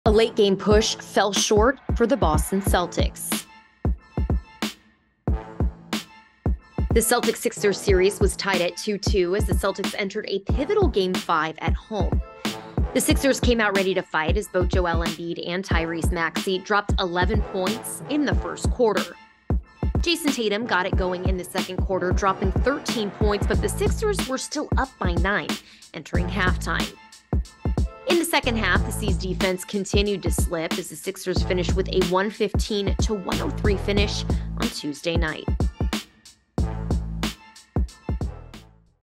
5/9 Putnam Postgame Report: Sixers Finish Strong
Joel Embiid and Tyrese Maxey didn’t allow the Celtics to get back into the game before closing out a 115-103 win.